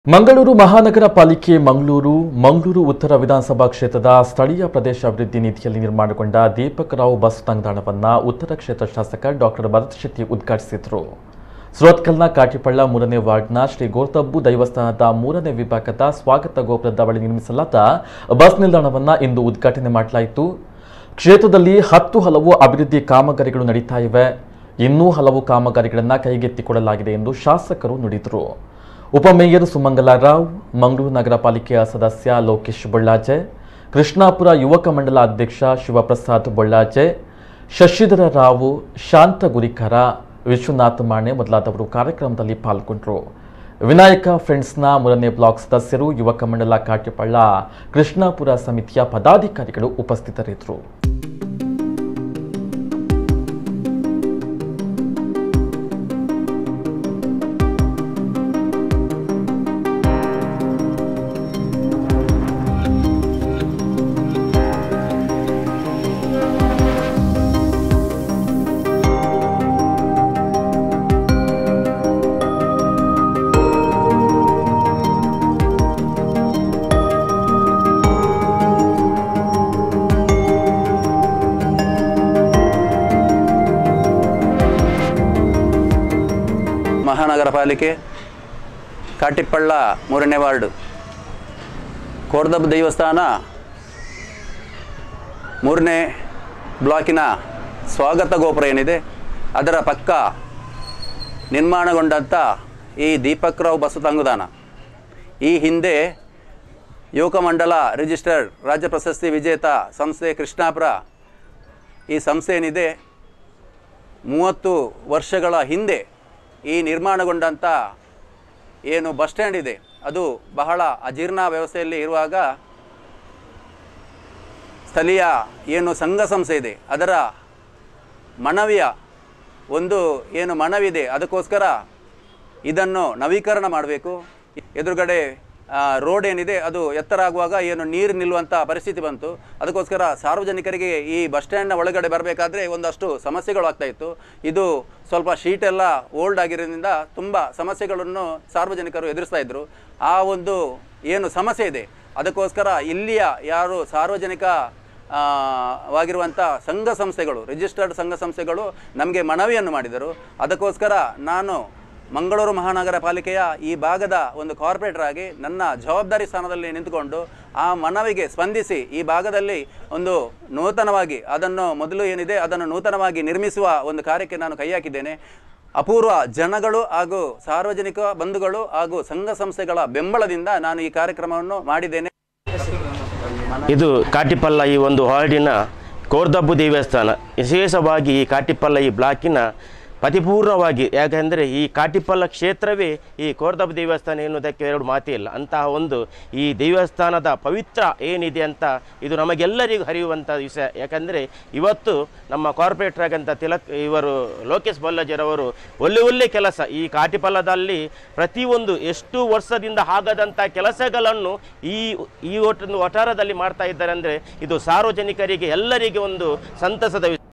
ಕಾಟಿಪಳ್ಳದಲ್ಲಿ ದೀಪಕ್ ರಾವ್‌ ಬಸ್ಸು ತಂಗುದಾಣ ಉದ್ಘಾಟನೆ
ಮಂಗಳೂರು ಮಹಾನಗರ ಪಾಲಿಕೆ ಮಂಗಳೂರು, ಮಂಗಳೂರು ಉತ್ತರ ವಿಧಾನ ಸಭಾ ಕ್ಷೇತ್ರದ ಸ್ಥಳೀಯ ಪ್ರದೇಶಾಭಿವೃದ್ಧಿ ನಿಧಿಯಲ್ಲಿ ನಿರ್ಮಾಣಗೊಂಡ ದೀಪಕ್‌ ರಾವ್‌ ಬಸ್ಸು ತಂಗುದಾಣವನ್ನು ಉತ್ತರ ಕ್ಷೇತ್ರ ಶಾಸಕ ಡಾ ಭರತ್‌ ಶೆಟ್ಟಿ ಉದ್ಘಾಟಿಸಿದರು. ಸುರತ್ಕಲ್‌ನ ಕಾಟಿಪಳ್ಳ ಮೂರನೇ ವಾರ್ಡಿನ ಶ್ರೀ ಕೋಡ್ದಬ್ಬು ದೈವಸ್ಥಾನದ ಮೂರನೇ ವಿಭಾಗದ ಸ್ವಾಗತ ಗೋಪುರದ ಬಳಿ ನಿರ್ಮಿಸಲಾದ ಬಸ್ಸು ನಿಲ್ದಾಣವನ್ನು ಇಂದು ಉದ್ಘಾಟಿಸಲಾಯಿತು. ಕ್ಷೇತ್ರದಲ್ಲಿ ಹತ್ತುಹಲವು ಅಭಿವೃದ್ಧಿ ಕಾಮಗಾರಿಗಳು ನಡೆಯುತ್ತಿವೆ. ಇನ್ನೂ ಹಲವು ಕಾಮಗಾರಿಗಳನ್ನು ಕೈಗೆತ್ತಿಕೊಳ್ಳಲಾಗಿದೆ ಎಂದು ಶಾಸಕರು ನುಡಿದರು. ಉಪಮೇಯರ್‌ ಸುಮಂಗಲಾ ರಾವ್‌, ಮಂಗಳೂರು ನಗರ ಪಾಲಿಕೆ ಸದಸ್ಯ ಲೋಕೇಶ್ ಬೊಳ್ಳಾಜೆ, ಕೃಷ್ಣಾಪುರ ಯುವಕ ಮಂಡಲ ಅಧ್ಯಕ್ಷ ಶಿವಪ್ರಸಾದ್‌ ಬೊಳ್ಳಾಜೆ, ಶಶಿಧರ ರಾವ್‌, ಶಾಂತ ಗುರಿಕಾರ, ವಿಶ್ವನಾಥ್ ಮಾನೆ ಮೊದಲಾದವರು ಕಾರ್ಯಕ್ರಮದಲ್ಲಿ ಪಾಲ್ಗೊಂಡರು. ವಿನಾಯಕ ಪ್ರೆಂಡ್ಸ್‌ನ 3ನೇ ಬ್ಲಾಕ್‌ ಸದಸ್ಯರು, ಯುವಕ ಮಂಡಲ ಕಾಟಿಪಳ್ಳ ಕೃಷ್ಣಾಪುರ ಸಮಿತಿ ಪದಾಧಿಕಾರಿಗಳು ಉಪಸ್ಥಿತರಿದ್ದರು.